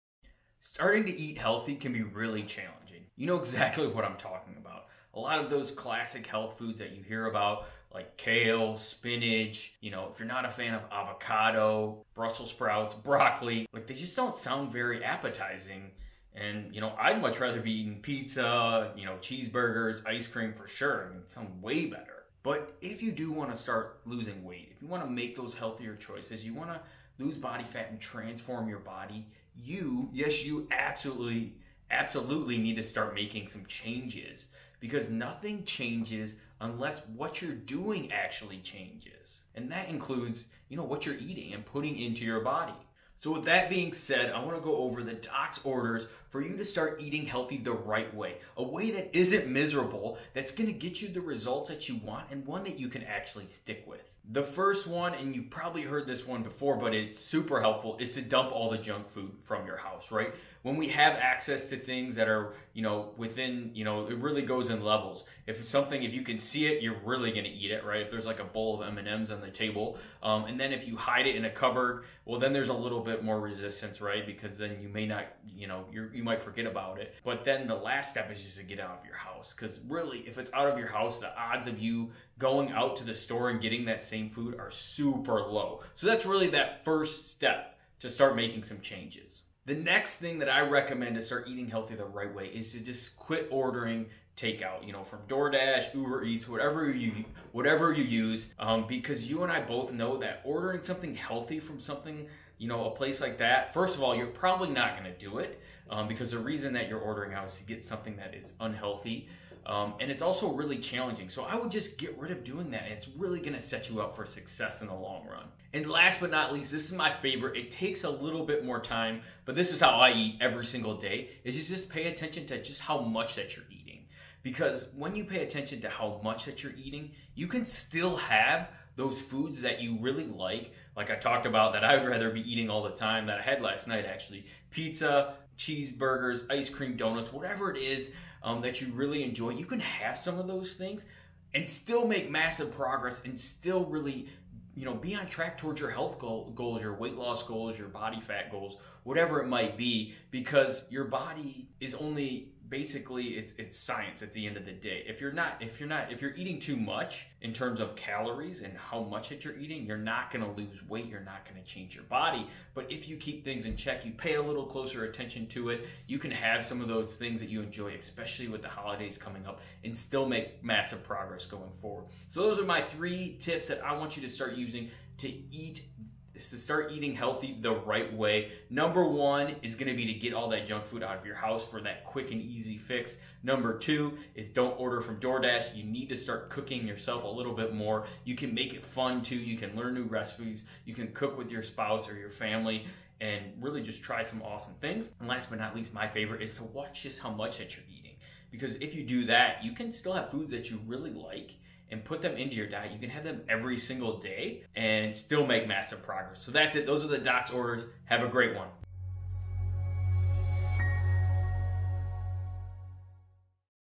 The Doc's Orders To Eat Healthy The Right Way
Still struggling to eat healthy?

With all of the different opinions out there on what you should eat to transform your health…

You may still be wondering what really works.

If that sounds like you…

Take a peek at today’s video where I go over exactly what you need to eat healthy the right way.